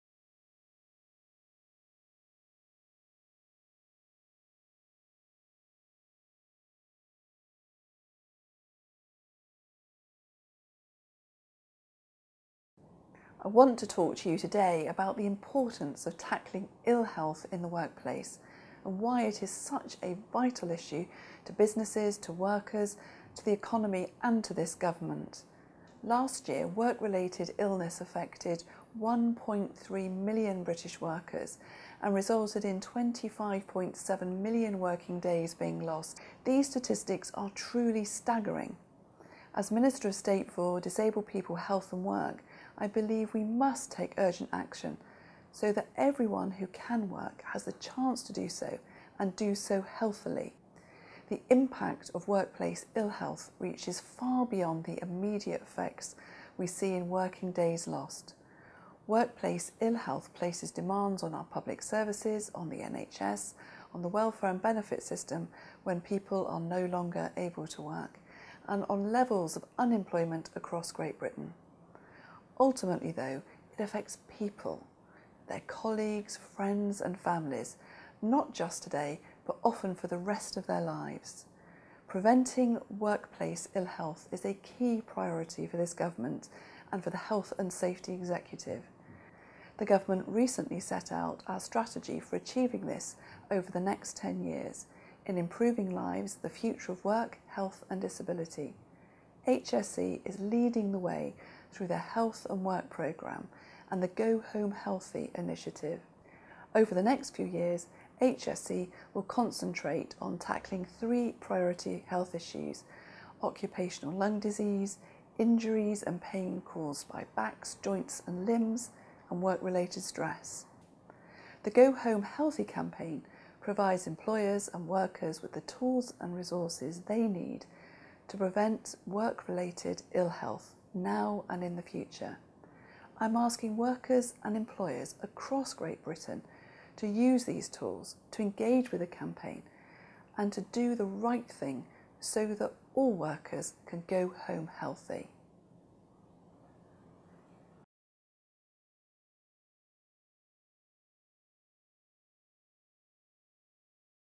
HSE's Minister asks you to engage with the Go Home Healthy campaign
Sarah Newton minister of state for disabled people, health and work, asks employers and workers to engage with the Go Home Healthy campaign to prevent work-related ill health